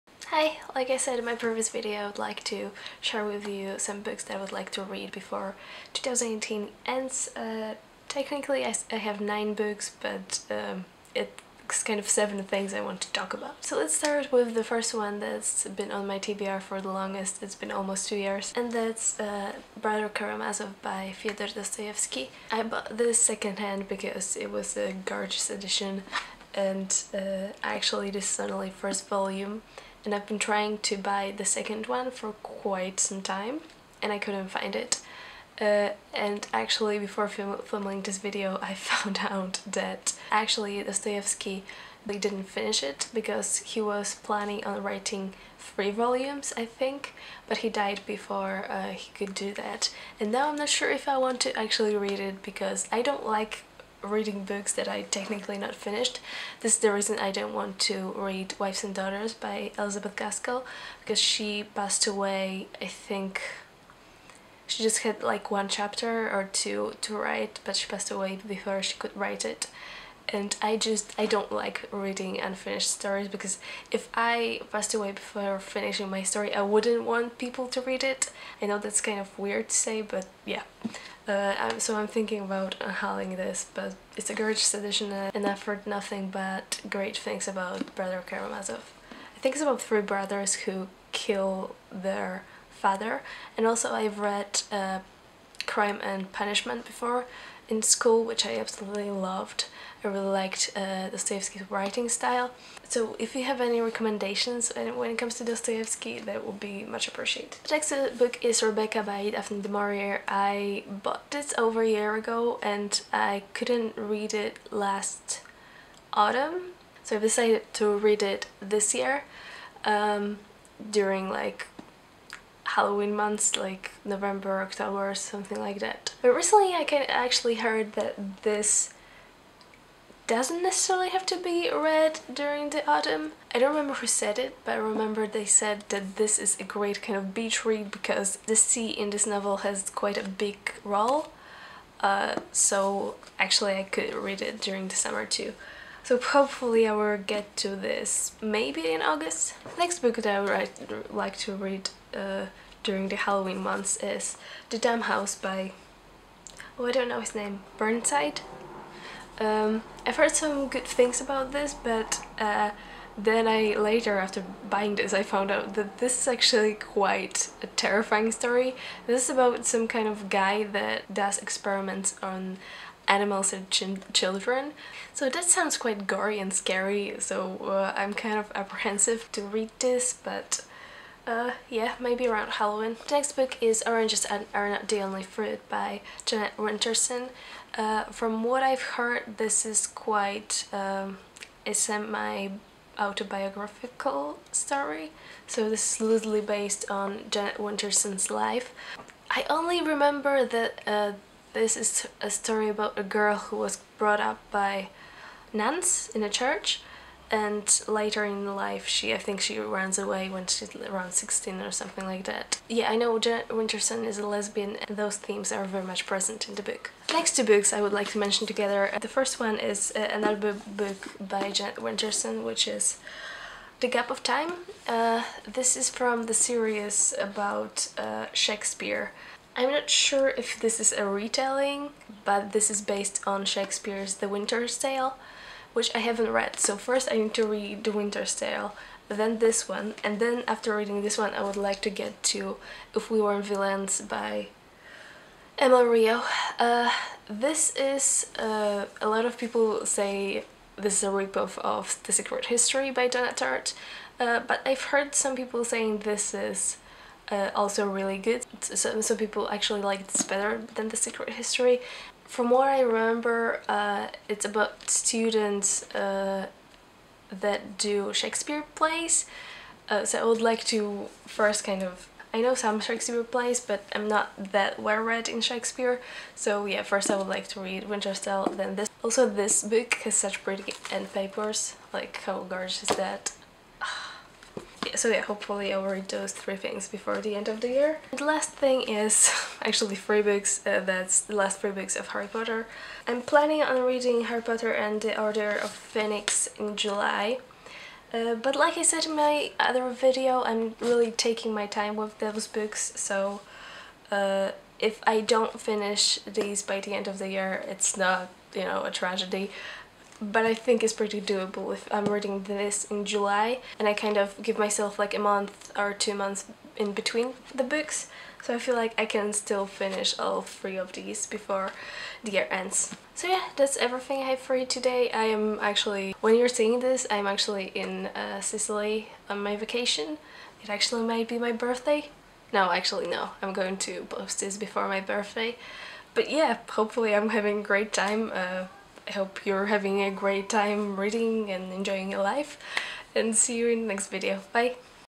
books I NEED to finish in 2018
a few things: OBVIOUSLY i can read Rebecca whenever? but since i've heard it's a gothic novel i wanted to read it in the autumn

also the section where i mention the gap of time and if we were villains is absolutely terrible and i make no sense whatsoever. i was jumping between different topics and i feel like the editing made it even worse

i'm a mess SORRY

books mentioned:

the brothers karamazov by fyodor dostoyevsky
rebecca by daphne de maurier
the dumb house by john burnside
oranges are not the only fruit by jeanette winterson
the gap of time by jeanette winterson
if we were villains by l.m. rio
harry potter and the order of phoenix